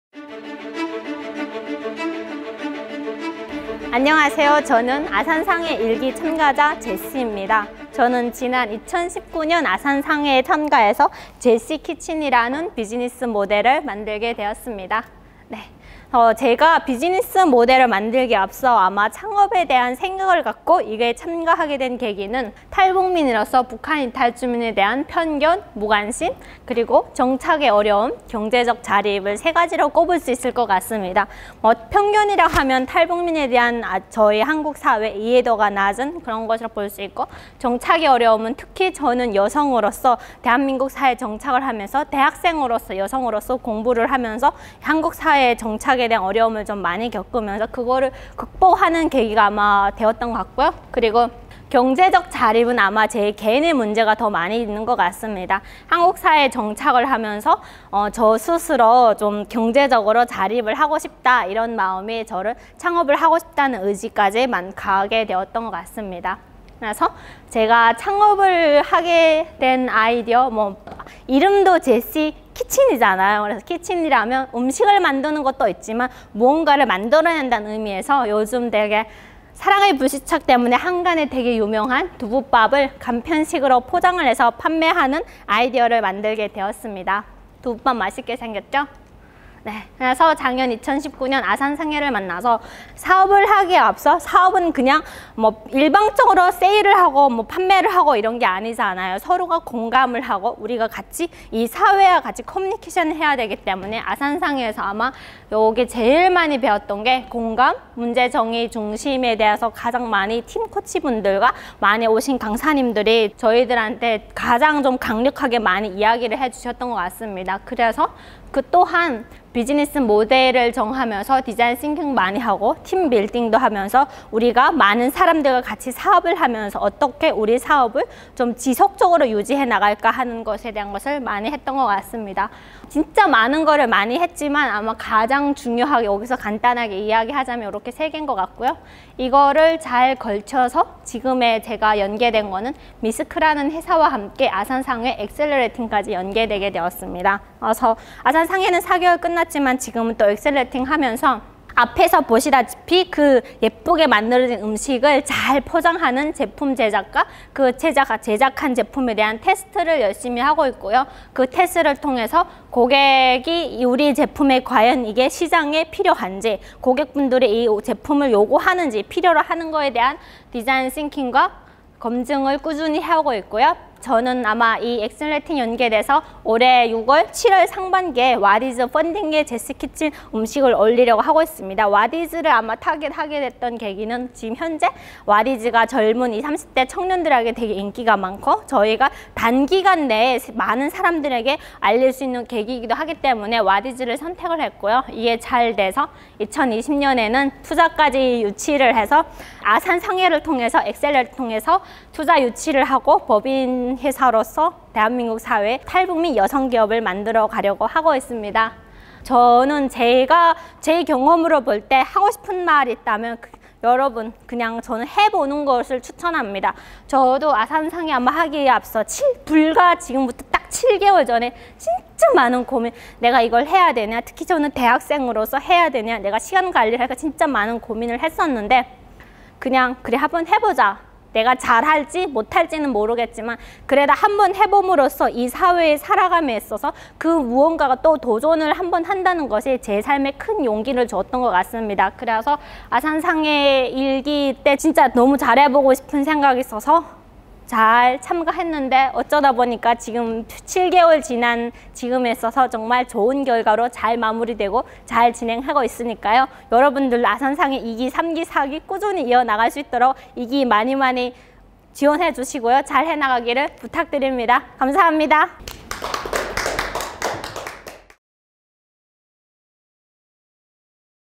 [아산나눔재단_ 아산상회] 1기 참가자, 지금은?_ 제시키친
아산나눔재단의 창업기회로부터 소외된 청년들의 창업을 지원하는 프로그램, 아산상회 2기 모집이 4월 23일까지 진행됩니다.
지난 아산상회 1기를 경함한 제시키친의 이야기를 만나보세요!

* '아산상회 무엇이든 물어보세요' 질문하기